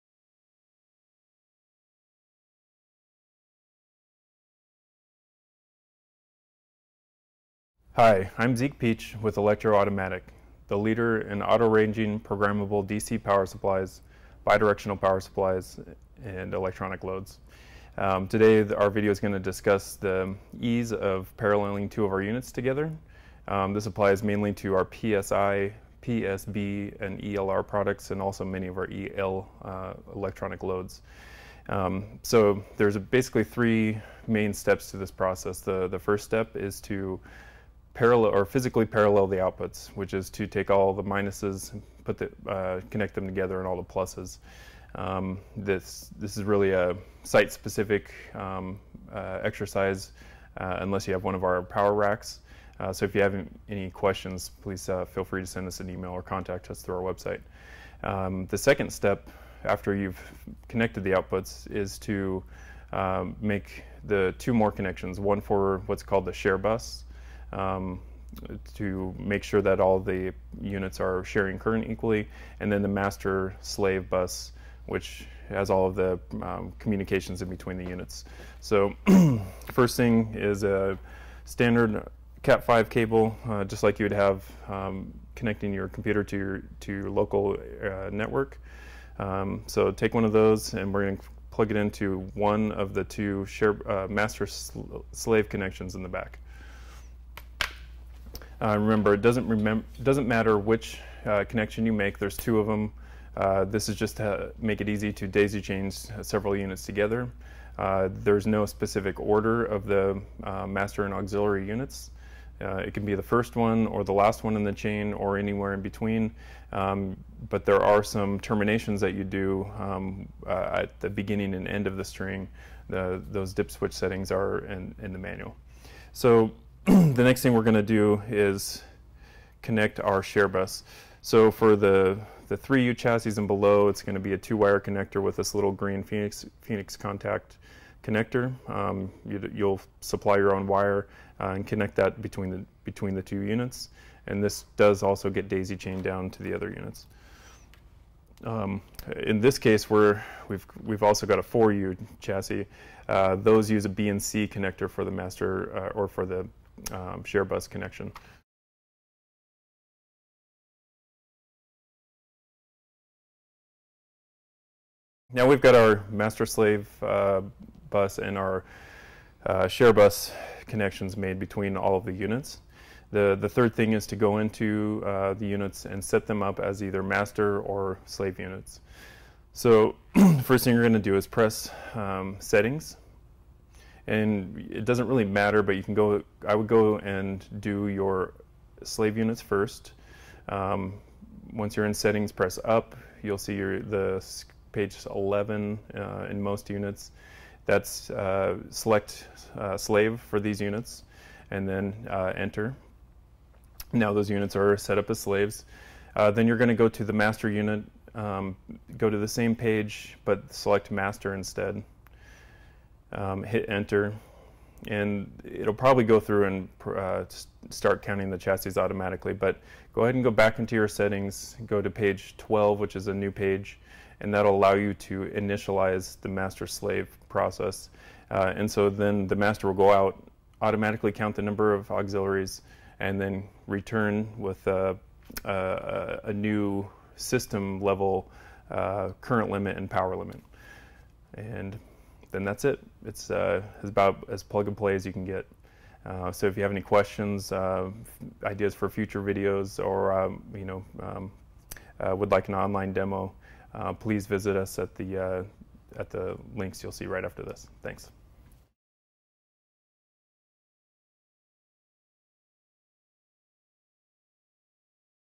Paralleling Power Supplies and Electronic Loads from EA Elektro-Automatik
How to parallel two programmable DC power supplies or electronic loads from EA Elektro-Automatik quickly and easily. for bidirectional and conventional programmable DC power supplies.

There are three steps to paralleling:
1 - physically parallel the +/- outputs
2 - connect the share bus to ensure units are sharing current equally
3 - connect the master/slave bus to ensure communication between the units

EA's units are easily paralleled via Cat-5 cable (for communication) and BNC cable (for current sharing). Watch the video to see the entire procedure and setup performed...

For state-of-the-art bidirectional DC programmable power supplies and regenerative electronic loads, visit us